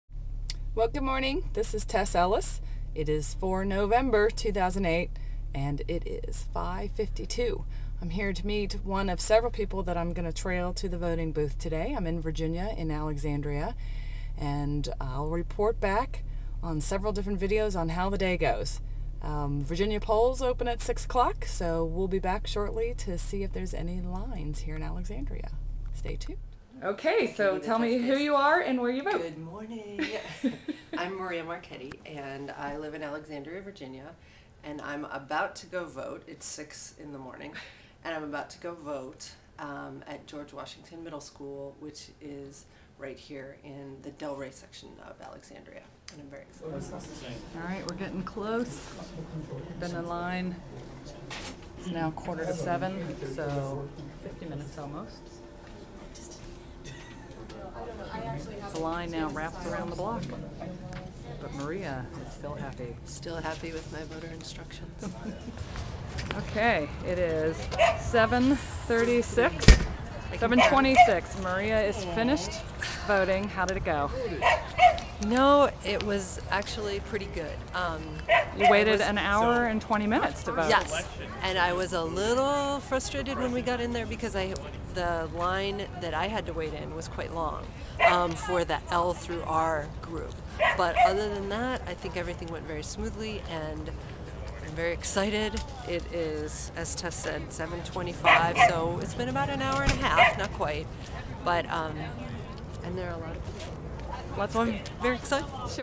Alexandria, VA early morning voting
Tess Ellis interviews Maria Marchetti in Alexandria, VA at George Washington Middle School in Del Ray at 6:00